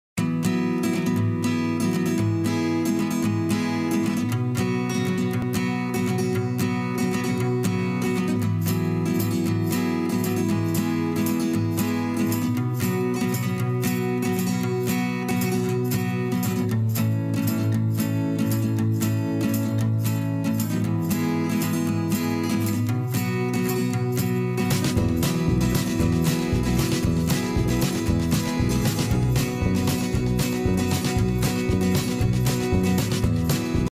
Chesapeake bay retriever leaping into water
Dezzy leaping into the water for her training duck. Dezzy is currently 8 months old.
The other dog is my Husky mix, Rocco. He tries to distract Dezzy but she stays focused.